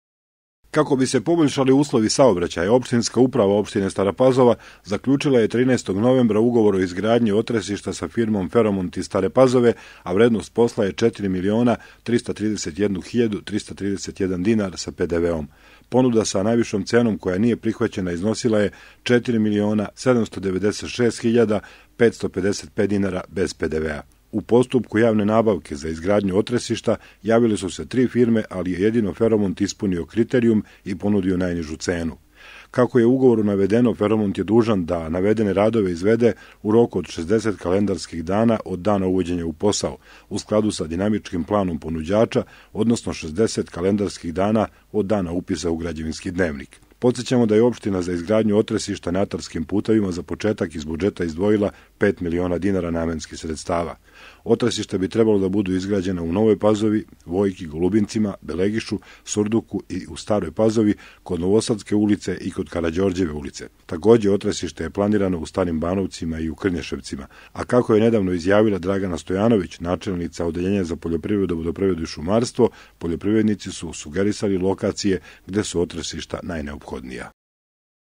Feromont gradi otresišta
U postupku javne nabavke za izbor izvođača radova za izgradnju otresišta na atarskim putevima, najpovoljnija ponuda stigla je od Feromonta iz Stare Pazove sa kojim je Opštinska uprava 13. novembra potpisala ugovor.